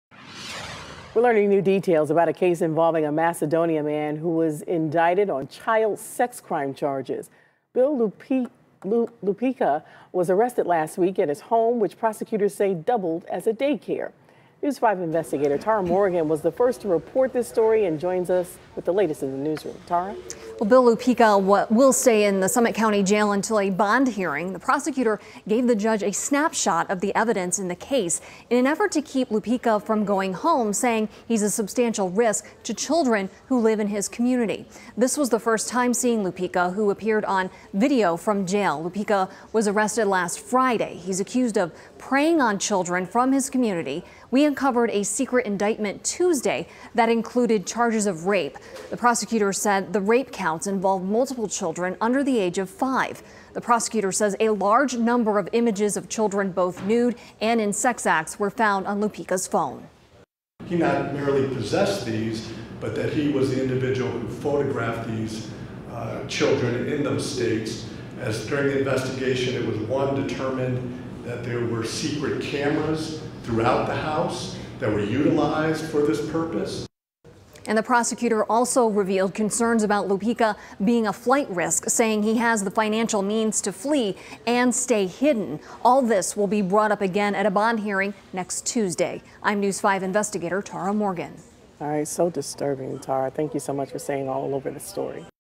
Graphic details revealed in case of Macedonia man accused of child sex crimes
Suspect appears in court for the first time